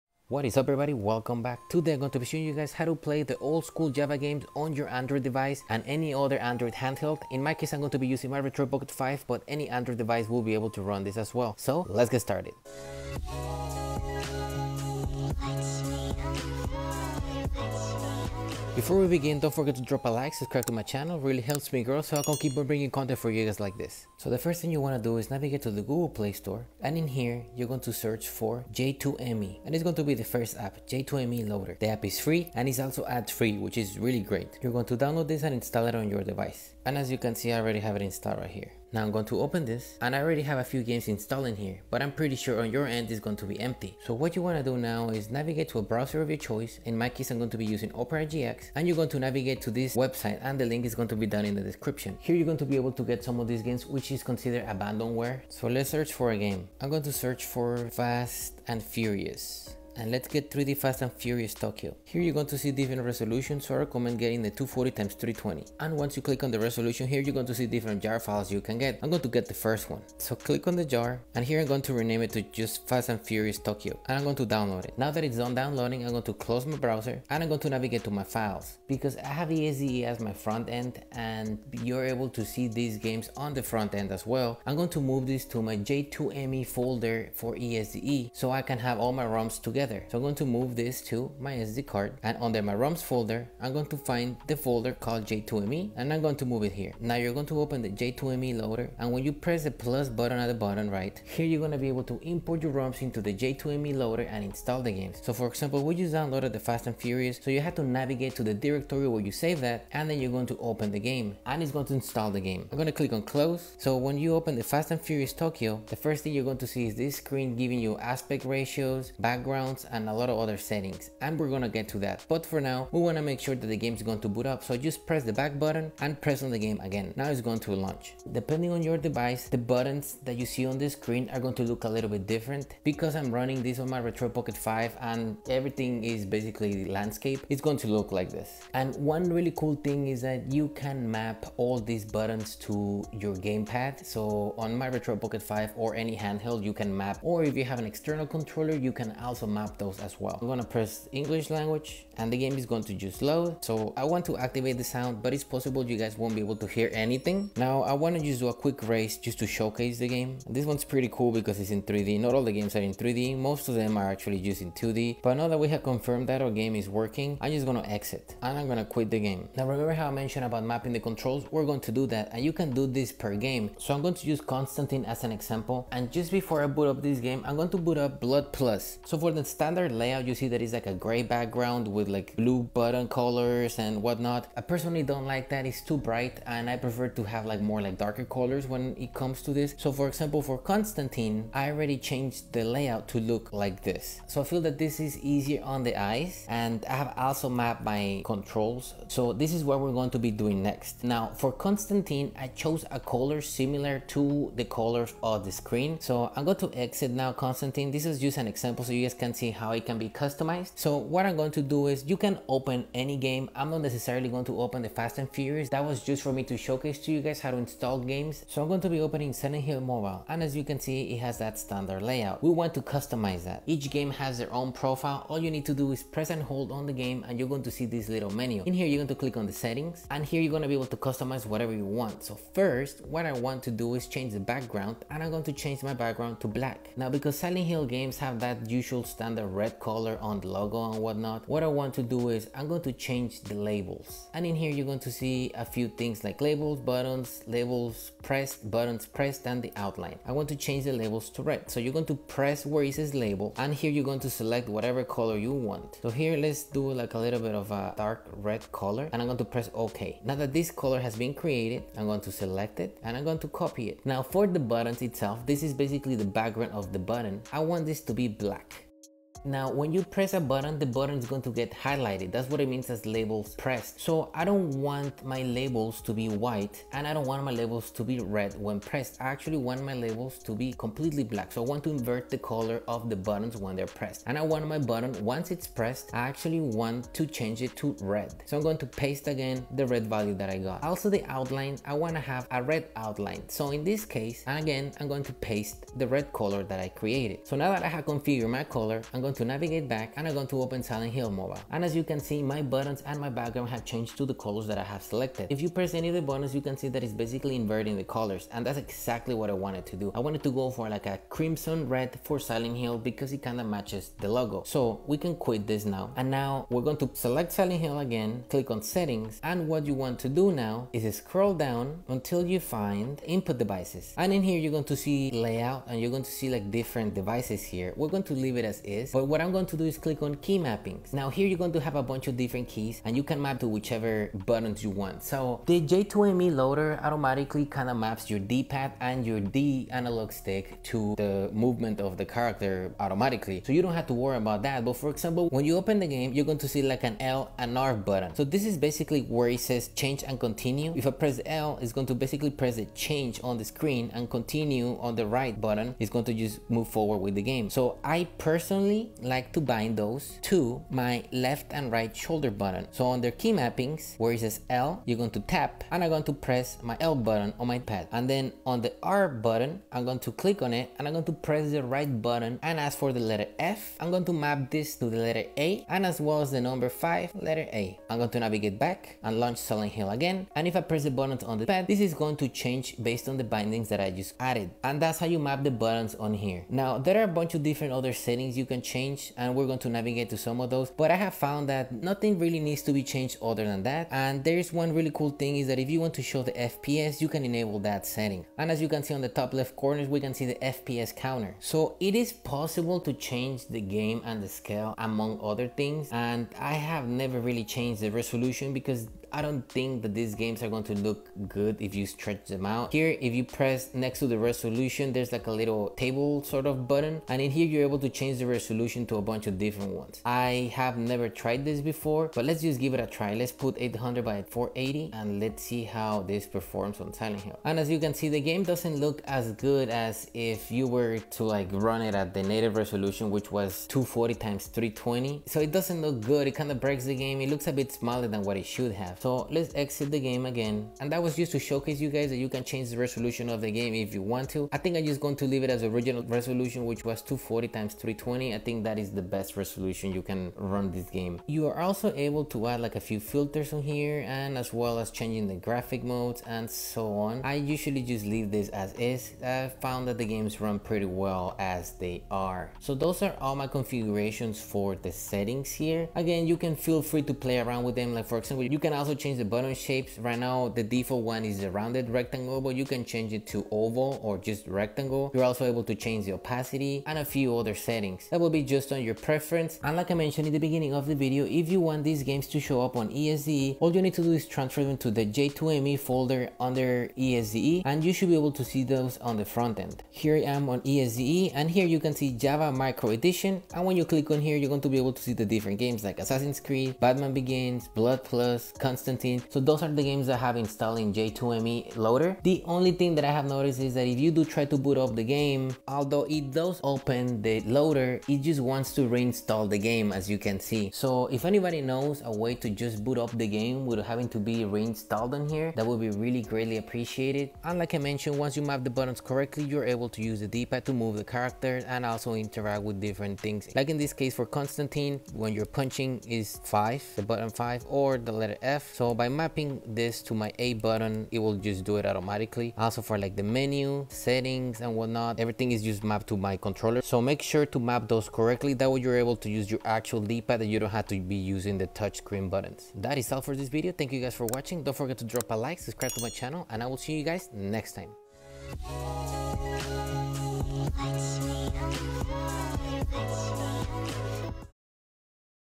J2ME Loader - Play Java Games on Android and Retroid Pocket 5 Quick Starter Guide!!!
In this video we are installing J2ME Loader, a Java Game emulator for android to play games on our devices. We are also testing out a few games, configuring the emulator, setting our key bindings, etc.